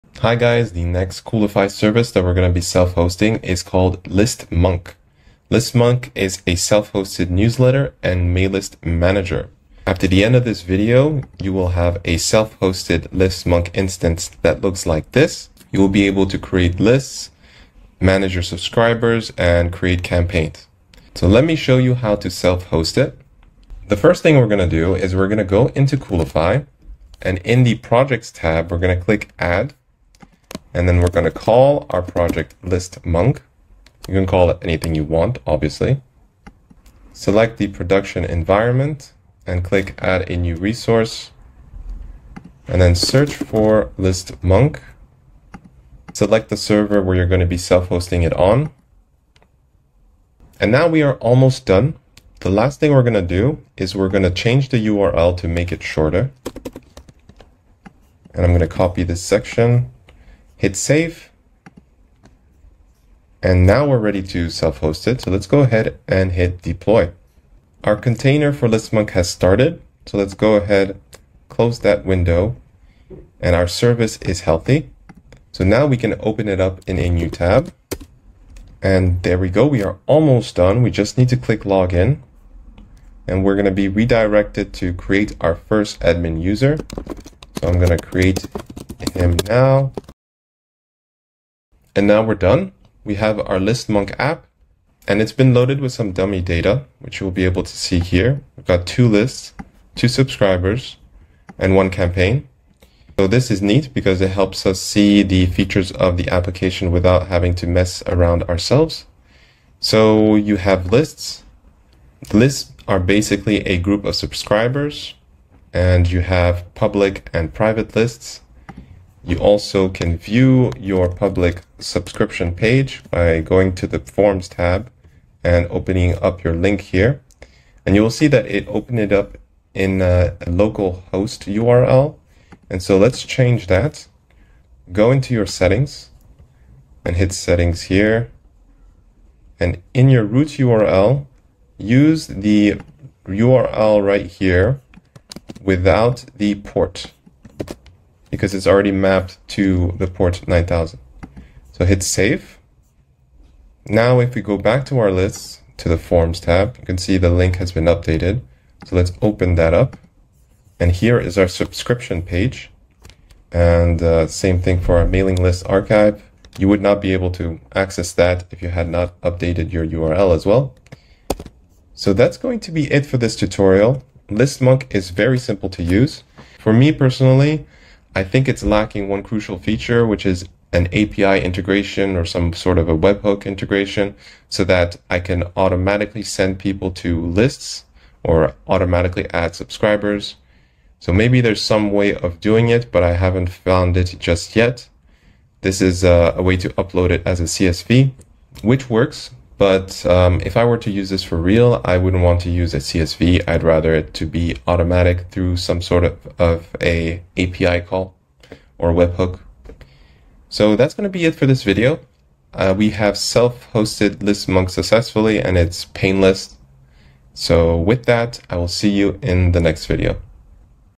How to Self host Listmonk on Coolify
Learn how to Self-host Listmonk and host a newsletter and mailing list manager using Coolify!

Coolify simplifies deployment and management of your apps. This video is part of a series where I try out all of Coolify's services.